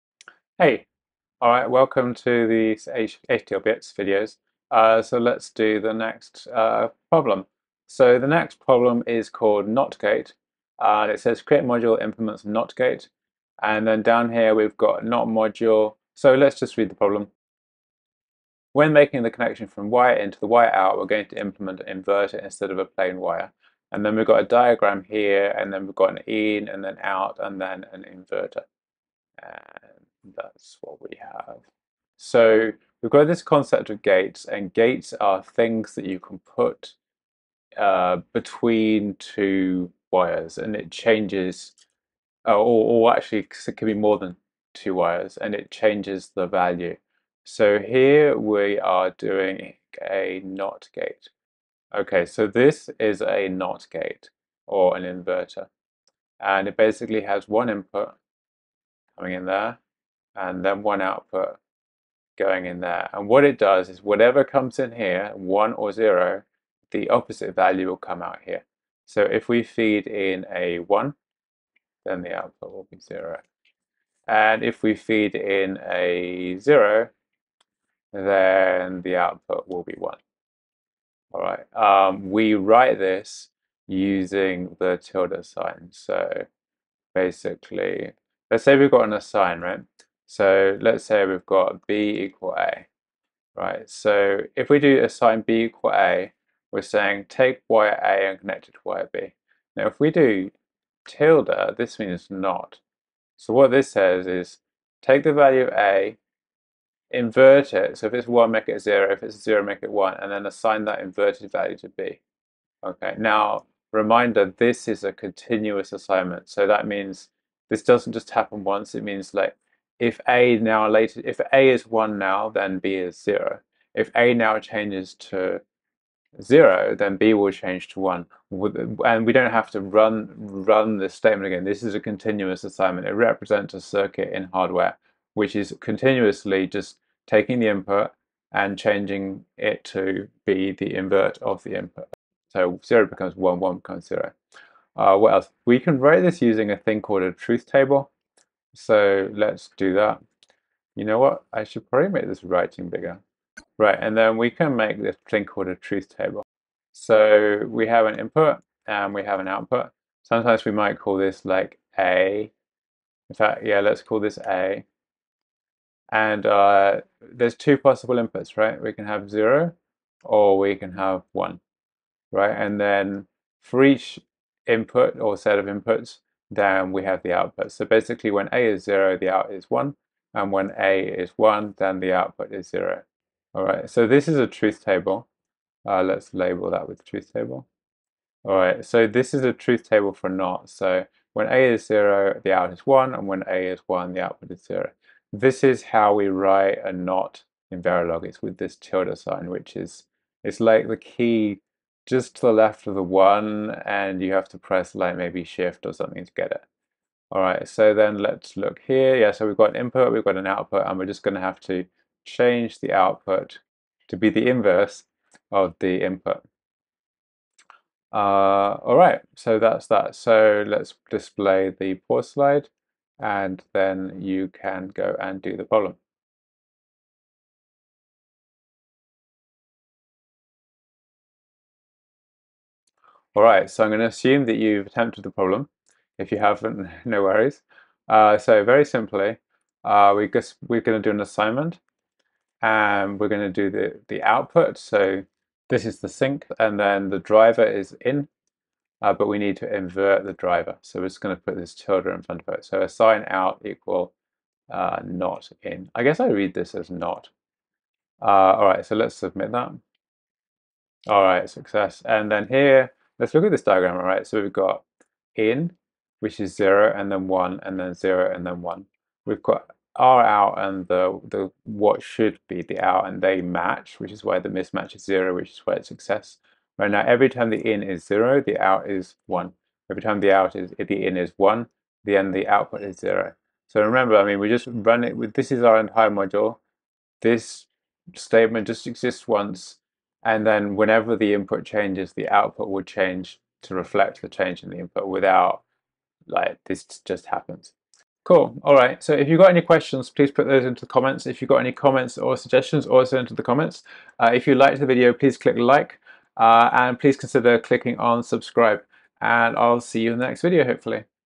Learn Verilog 3: NOT gate
We will use the problems at hdlbits, , in order to learn Verilog. In this video we cover ‘not’ gates.

Problems in this video:
- Notgate

Verilog is a language for hardware chip design. It is used to design silicon chips such as GPUs and CPUS.

Content:
00:00 - Notgate problem
00:32 - Notgate background
03:52 - Solution to Notegate problem
04:58 - Outro